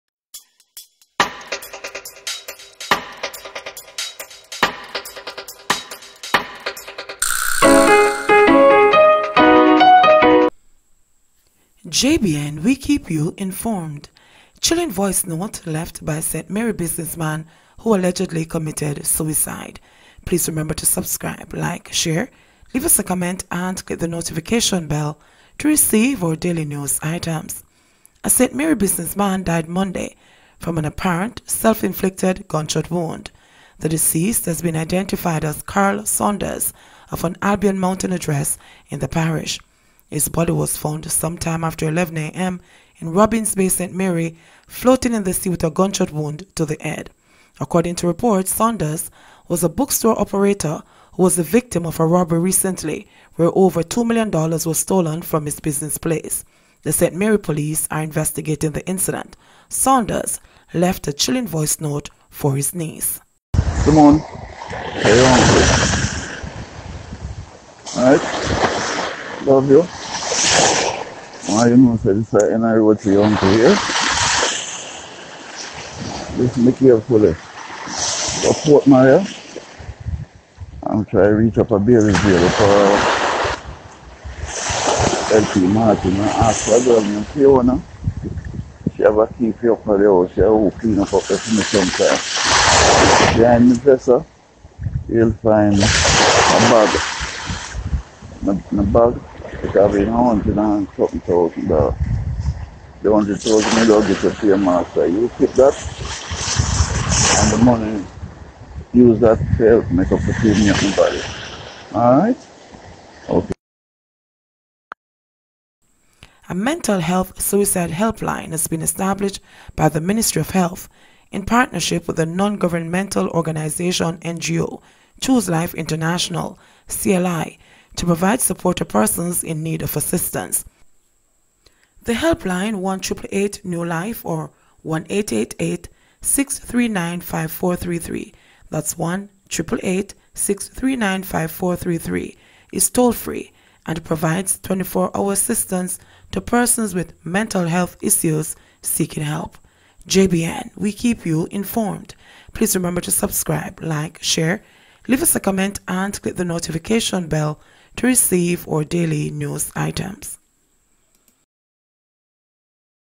Ch!lling Voice Note Left By St Mary Businessman Who Allegedly Comm!ted Sv!c!de/JBNN
A St Mary businessman died Monday from an apparent s3lf-inflicted gvnshot wound.

The deceased has been identified as Carl Saunders of an Albion Mountain address in the parish.